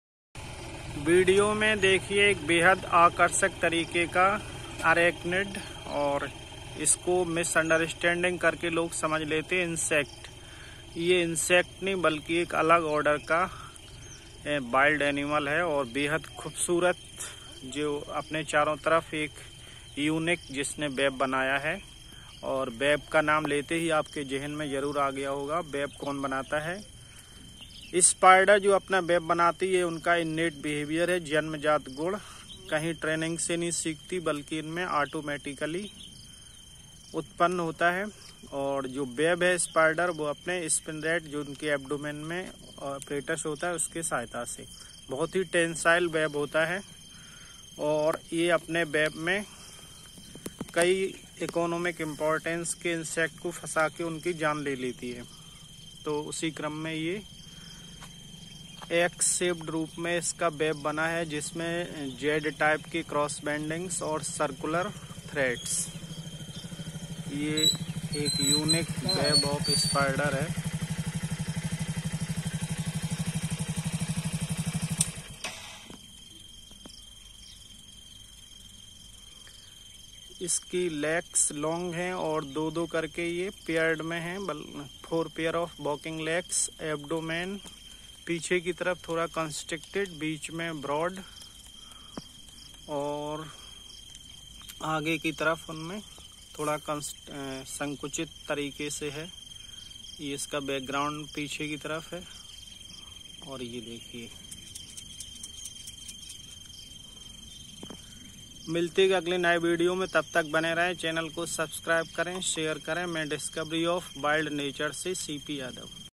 Writing spider or Black yellow garden spider of or web spider Argiope suja
Rarely found and mostly occurrence in denses herbs and shrubs
Female is about 8–12 mm long and male is 3.5-4.5 mm. After Cephalothorax greyish brown with hairs. Sternum heart shaped with hairy pubescent white patch. Palps bear spines. Legs greyish brown and hairy. Femora dorsally yellowish. Abdomen pentagonal and hairy. Dorsum yellowish with brown transverse bands. Three sigilla pairs distinct. Ventrum dark brownish with two longitudinal white patches.

Like other species of the same genu is known as a "signature spider"; it builds a web with a zig-zag stabilimentum somewhat resembling letters of english alphabet 'X' The mature female of A. anasuja always rests at the centre of the orb with her head facing downwards. Legs are expanded in both direction of body backward and forwards  legs are long.The orb has an opening at the centre and when disturbed she goes through the hole and exits on the other side of the plane of the web.
Web also used for wating  for prey,and capturing its essay to capture and also escaping from their enemies
Sexual diamorphism also clear male ia smaller in size in comparison to female